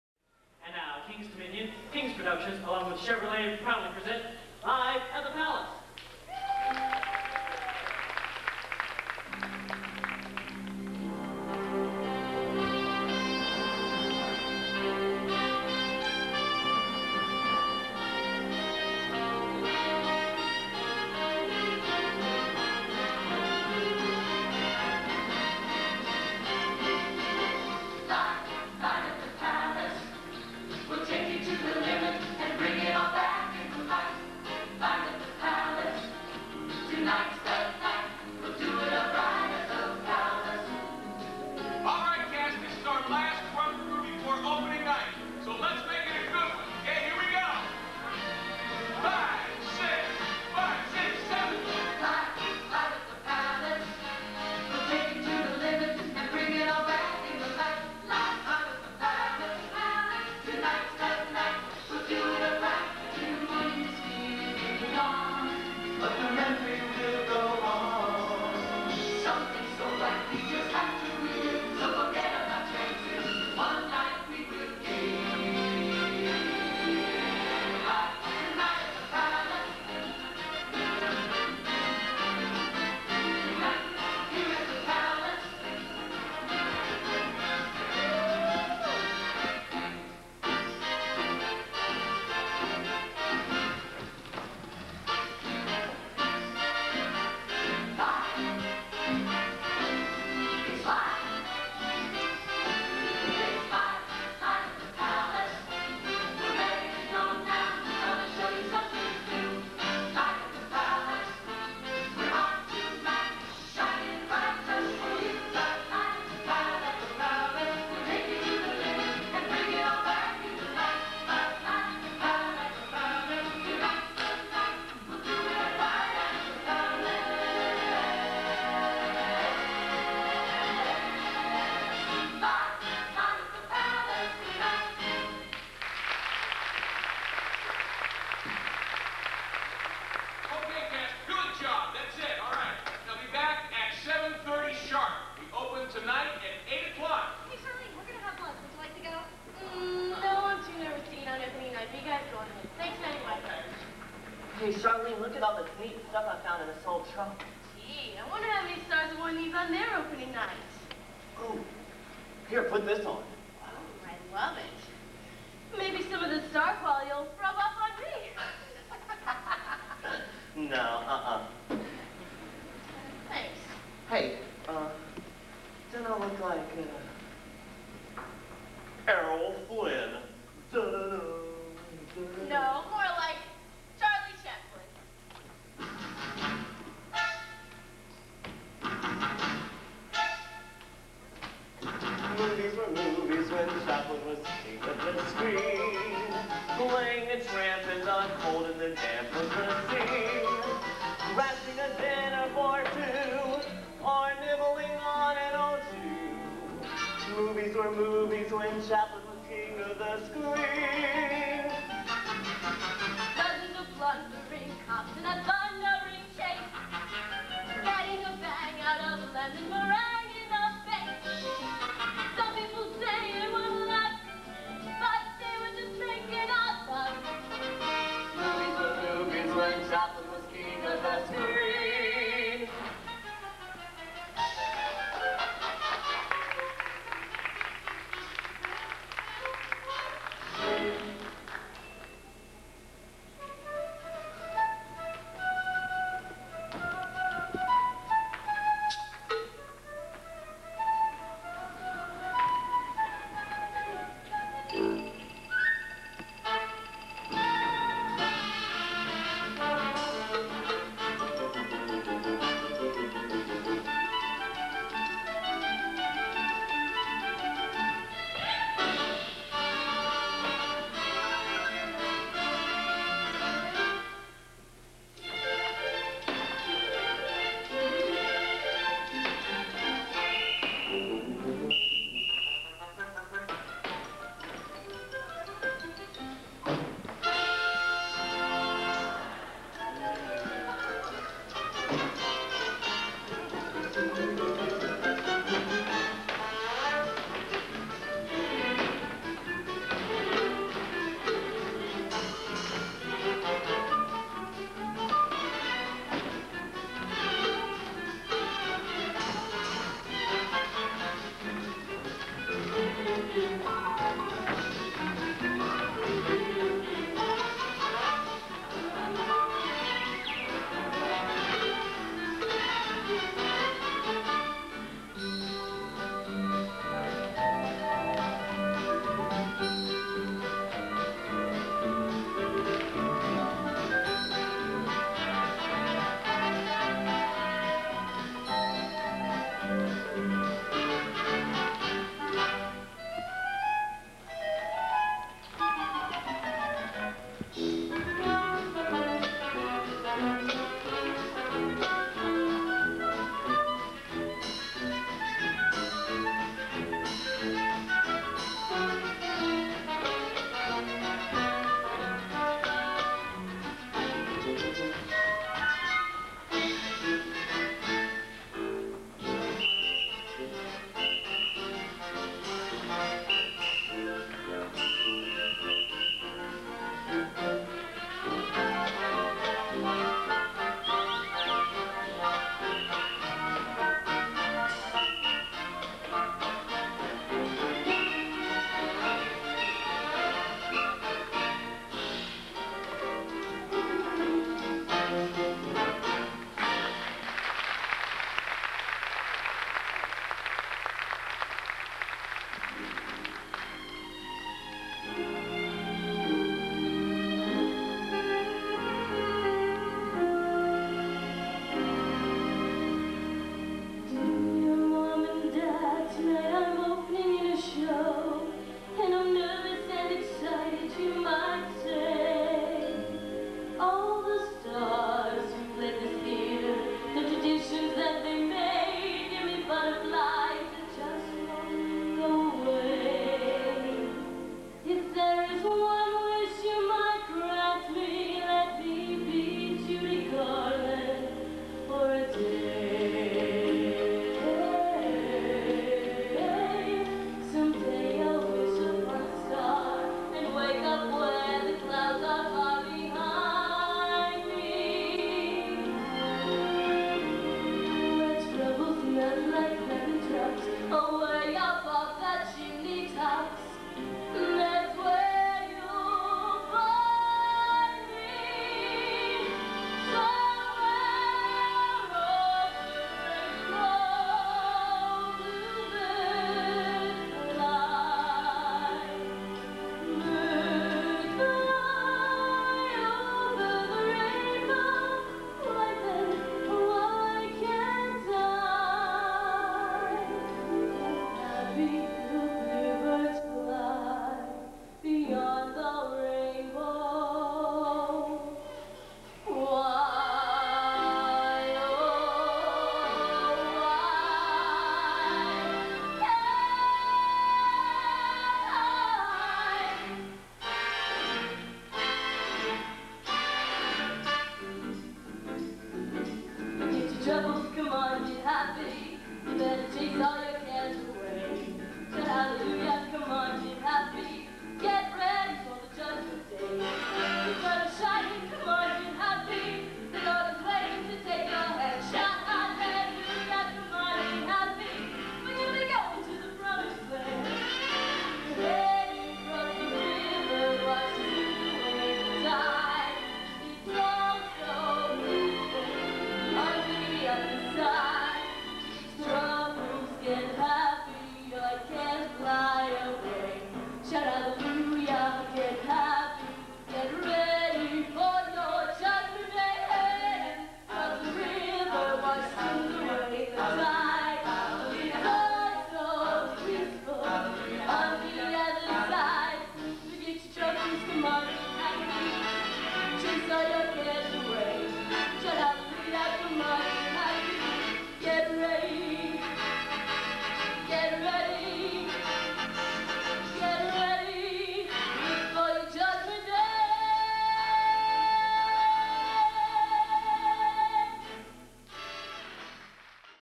Live at the Palace part 1, Kings Dominion
Live at the Palace was a major show that ran at the main theater of Kings Dominion in 1990.  The show is to give a back stage look at a cast that is getting ready for opening night.  The show had a 16 member cast and had major set changes as well as costume changes and ran for a total of 30 minutes about. You will find the full show uncut on my sight in 3 parts.  Please be sure to look for all of these and also for other shows that ran at Kings Dominion in the 1990's as well as other parks.  This was the first time I ever recorded a show and I was using a friends video camera for this taping.  I hope you will enjoy the show!